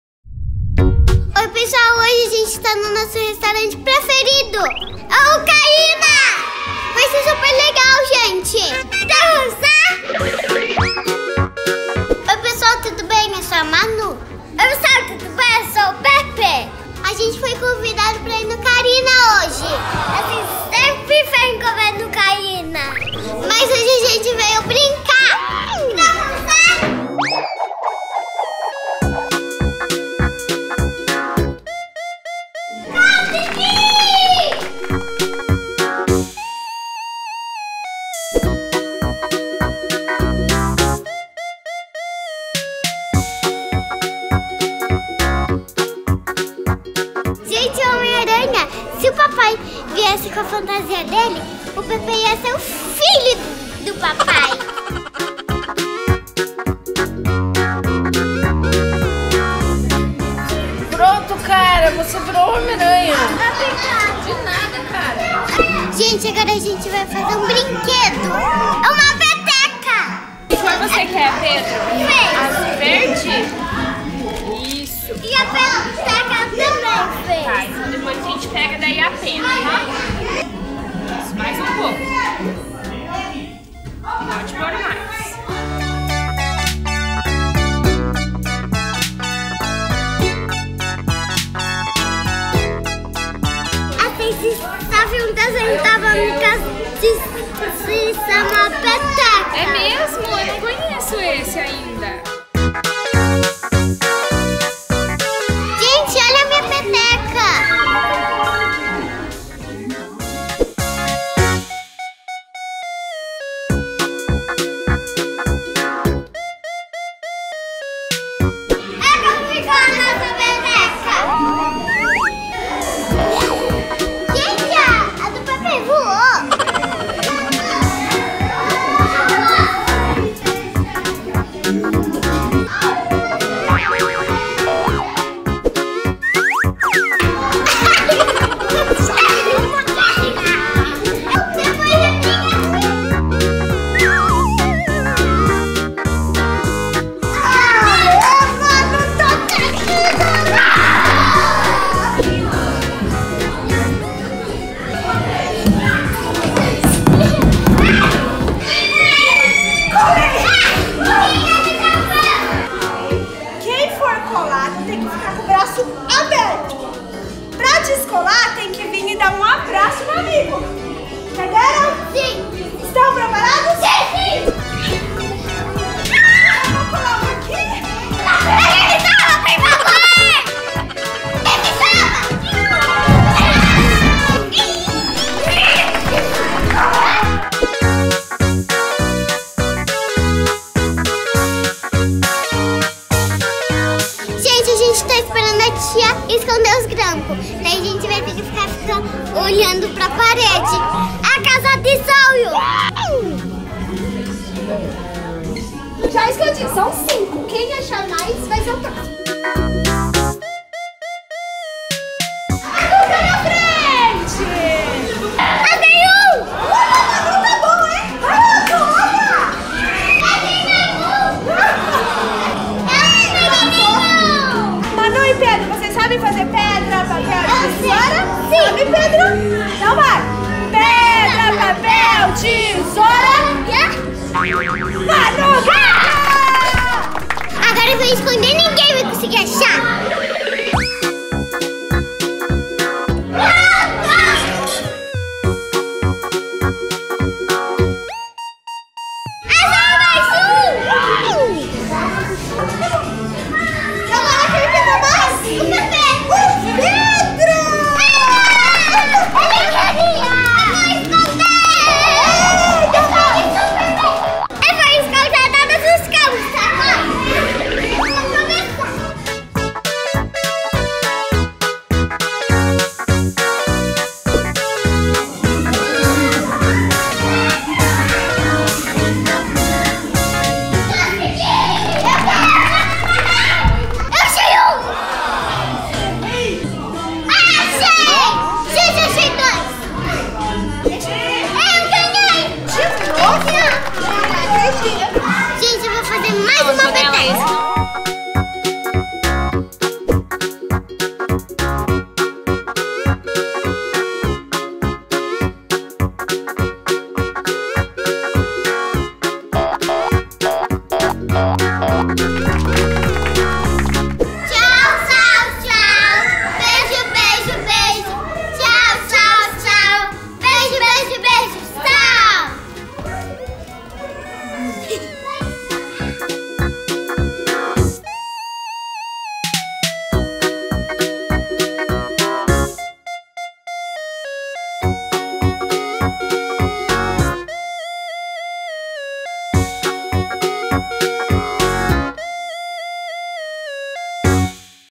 Passeio no Kharina do Cabral
Manu e Pepê foram convidados para brincar no Restaurante preferido deles: Kharina do Cabral.
Lá a Equipe da MUV animou todas as crianças presentes.
E lógico que não faltou o Milk Shake de sobremesa.

Cartinhas & Presentes:

📦 Caixa Postal 4031 CEP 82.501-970 Curitiba / PR

👉 Canal Infantil: Livre para todas as idades, administrado e monitorado pelos responsáveis.

❤ Agradecimentos:
Kharina Cabral
Equipe MUV